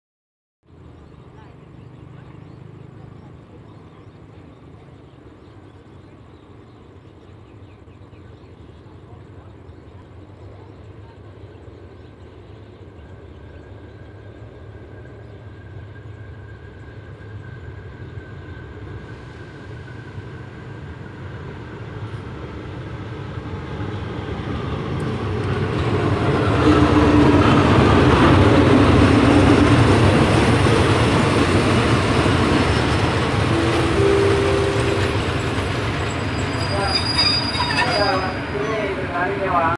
GEA4525
รถด่วนข.86นครศรีธรรมราช-บางบำหรุ
เข้าจอดสถานีรถไฟฉวาง